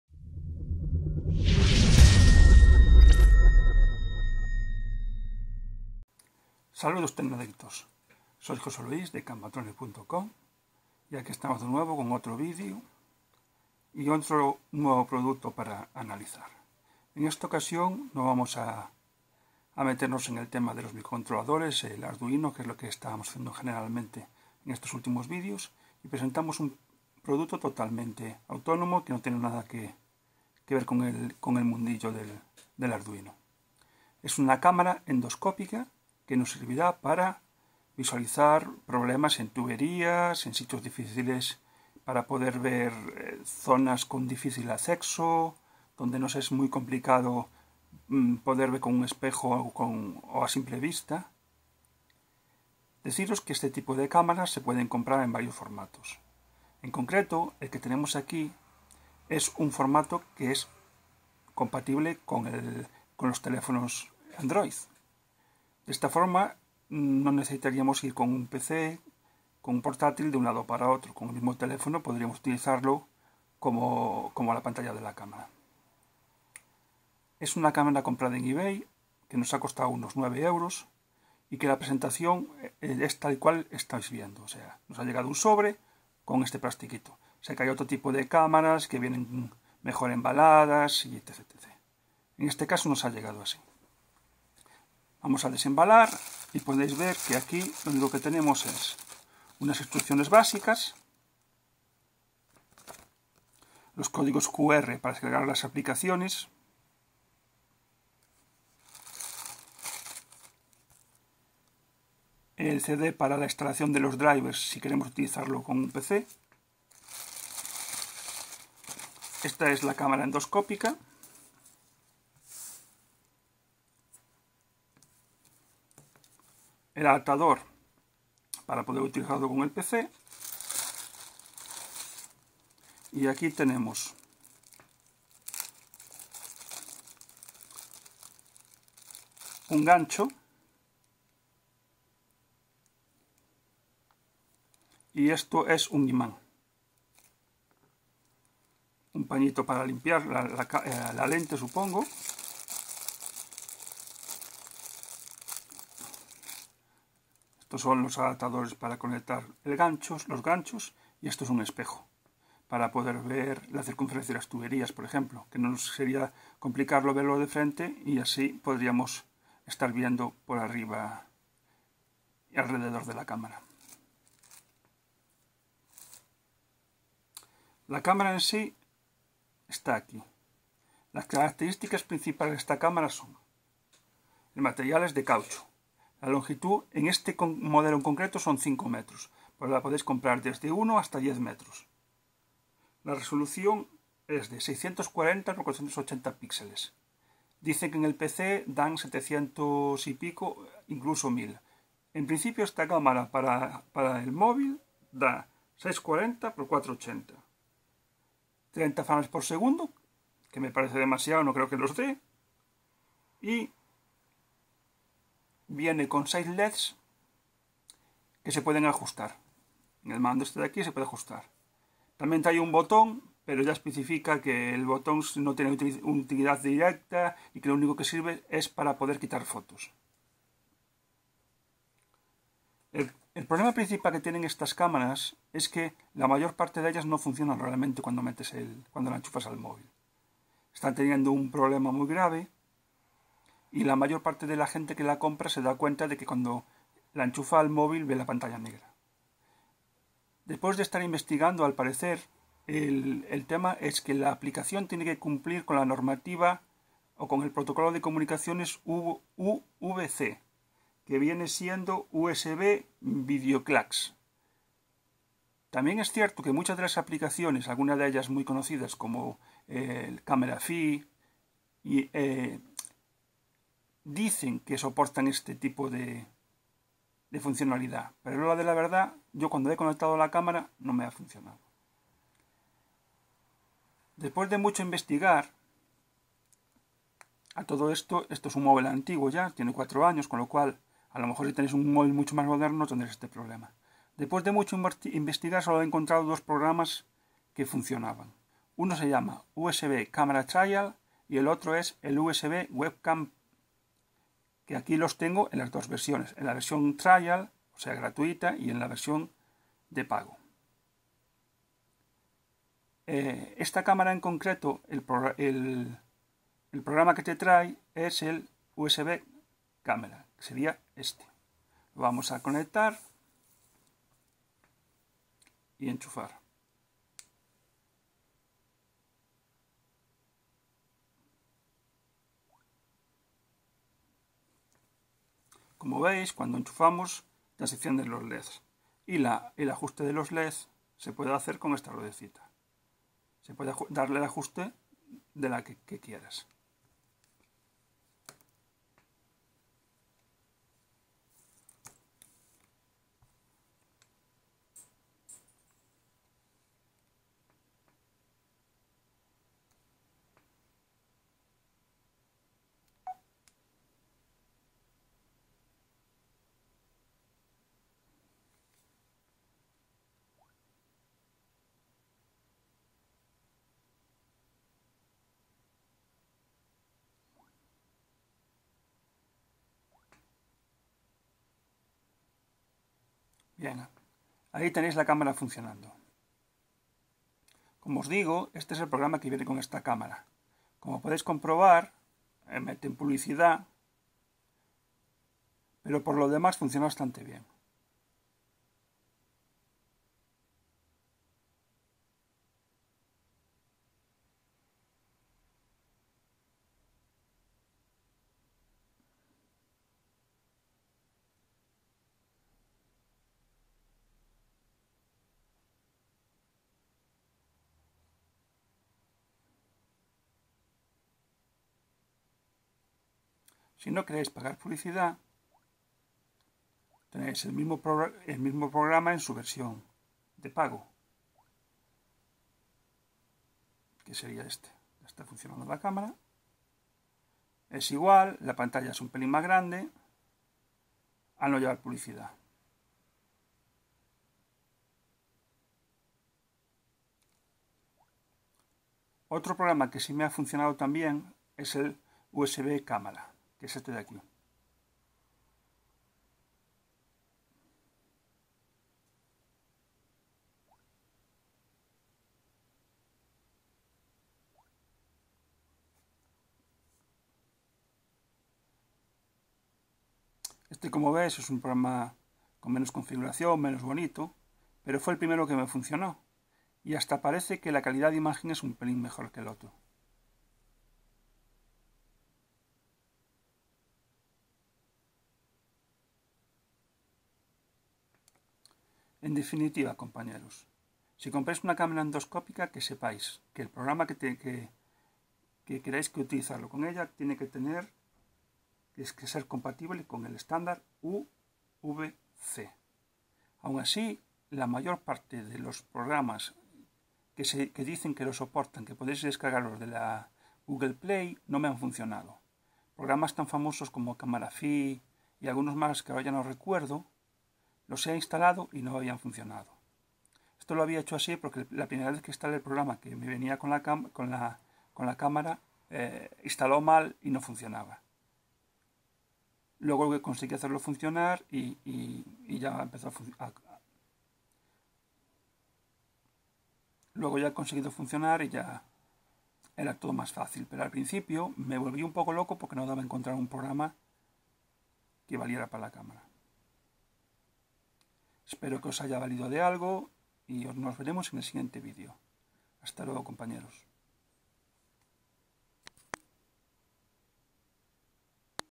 Revision Camara Endoscopica 5 metros con leds.
Estamos ante una cámara endoscópica que se puede utilizar para revisar tuberías, colocar entre paredes, meter por agujeros y demás. Tiene 5 metros de cable y es conectable a smartphone android.